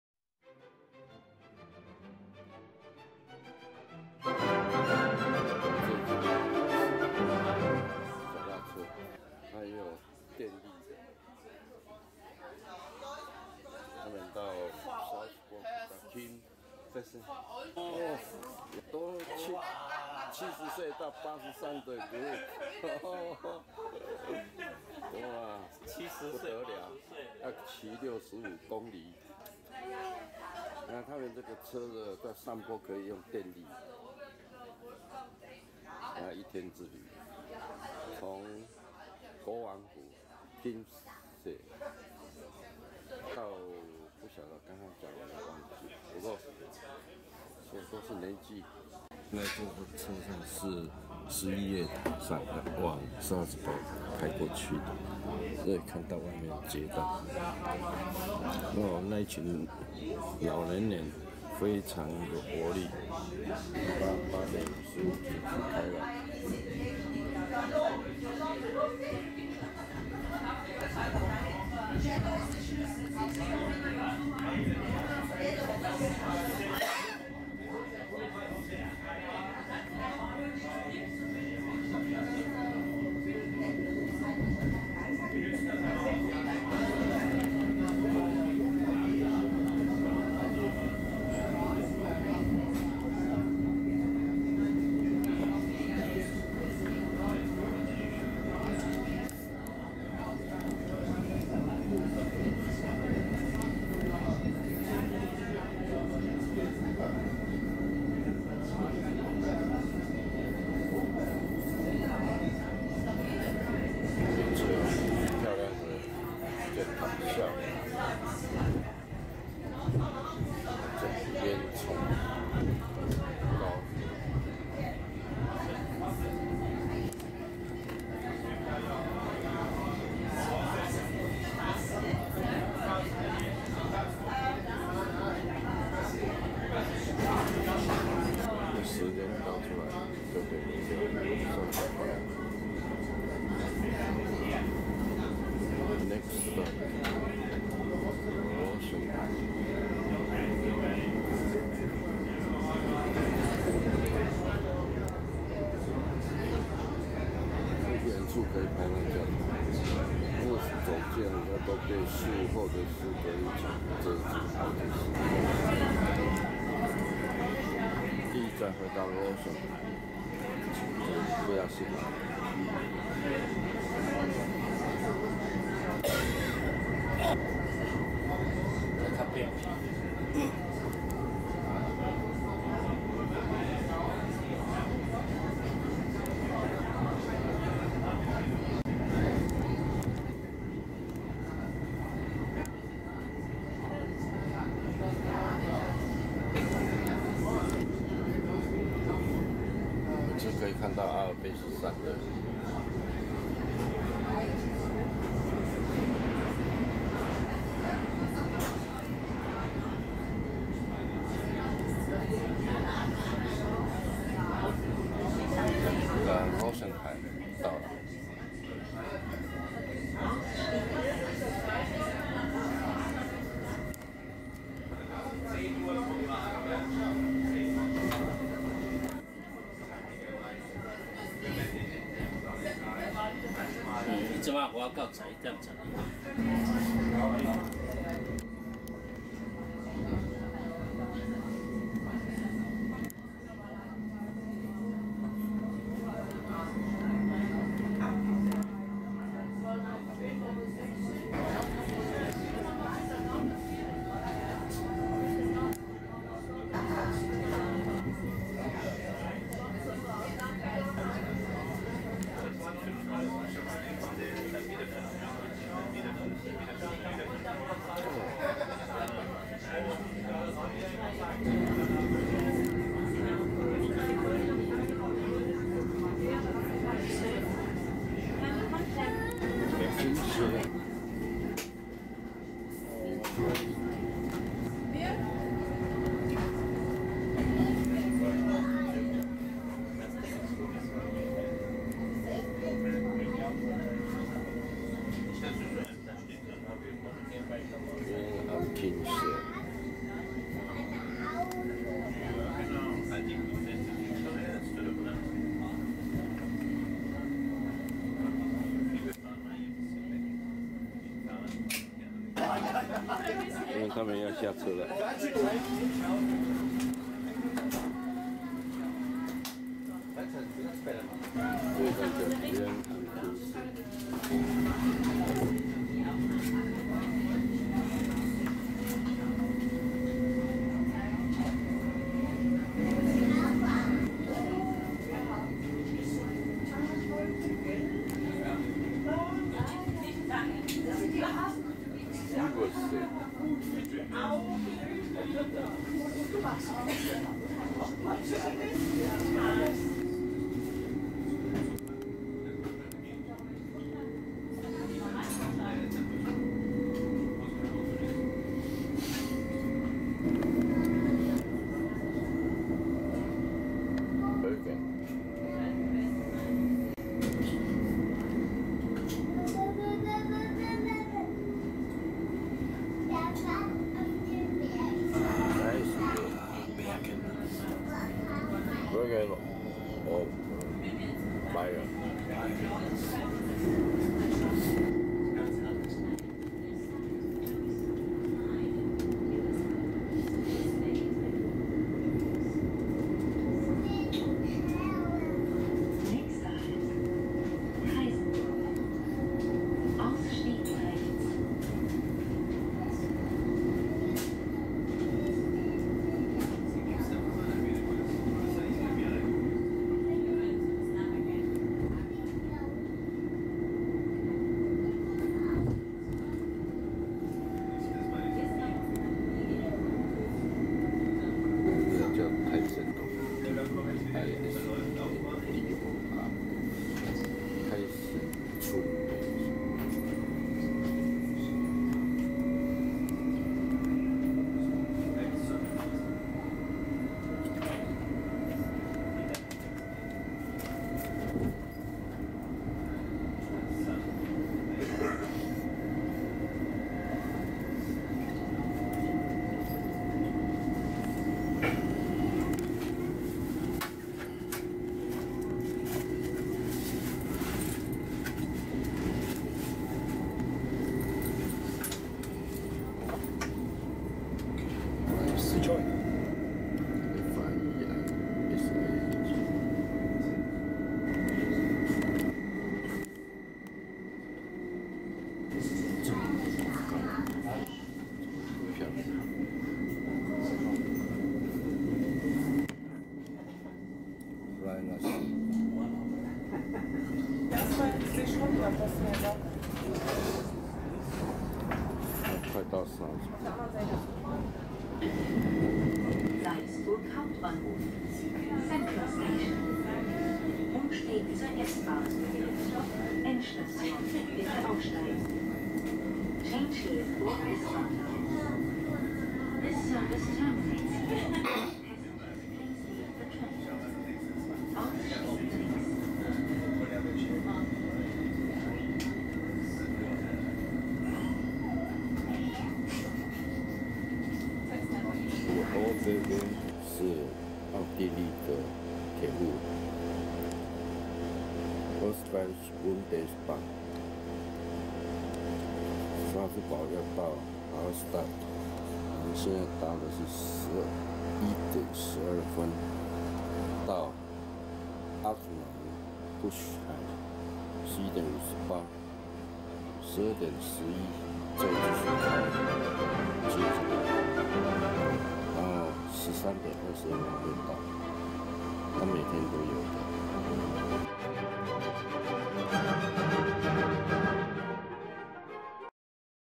Munich to Salzburg by train 慕尼黑到薩爾茲堡火車行
一群人的年紀,從70歲到83歲不等,但他們非常有活力
The age of these people varies from 70 to 83 but they are very energetic
他們將要騎65公里的路程
They will ride 65 kilometers away
火車經過有湖泊的小城,遠處能看到阿爾卑斯山
The train passes by the small town with a lake and the Alps can be seen in the distance